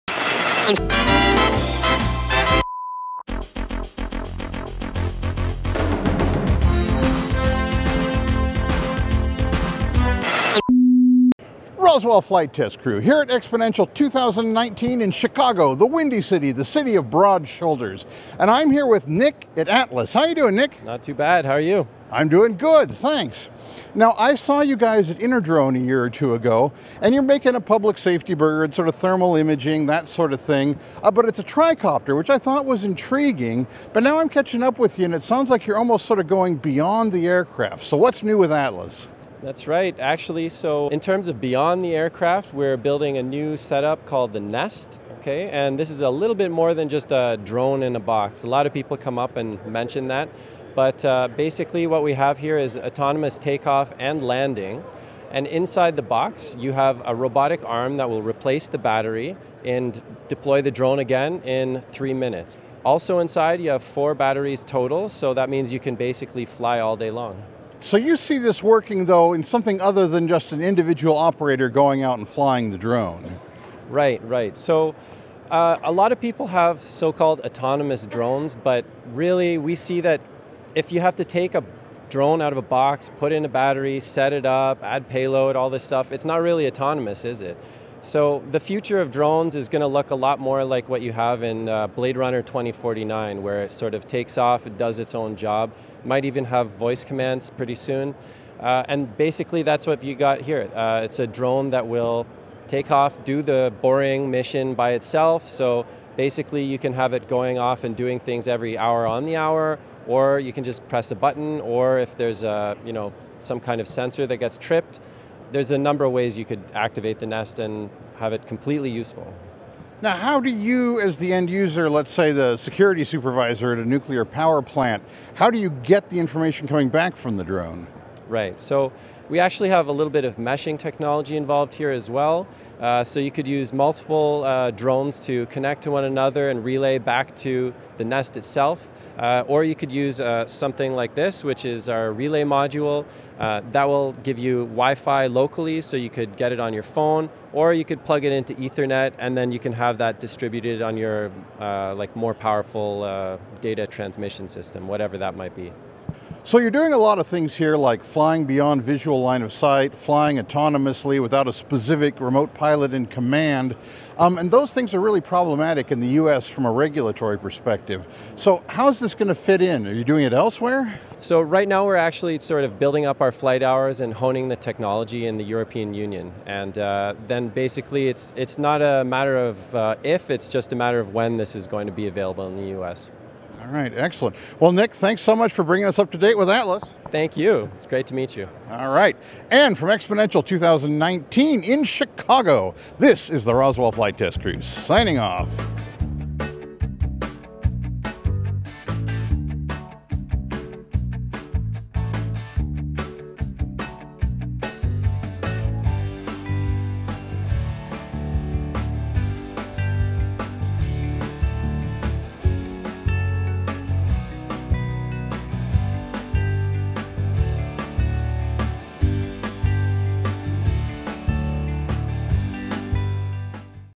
Atlas Dynamics: Autonomous Patrol Drone (not OK with FAA)
In this episode, the Roswell Flight Test Crew talks with Nikolas Trutiak of Atlas Aerospace about the company's tricopter-configured drone for public safety and other security applications. The aircraft works with the Nest: a weatherproof housing that recharges and replaces aircraft batteries automatically. It contains four batteries, which can be replaced in as little as three minutes, meaning it is capable of sustained flight operations. The drone can take off from and land in the Nest autonomously. With appropriate authorizations, it is capable of flying perimeter patrol missions under its own control, as well as responding to alarms. The drones are capable of forming a mesh network with each other and the Nest, as well as a hand-held repeater and data downlink. To reach its full potential, the Atlas require fully autonomous and Beyond Visual Line of Sight (BVLOS) operations, which are difficult to achieve under current Federal Aviation Administration (FAA) regulations. Consequently, Atlas is currently flying in the European Union to test and prove its technology.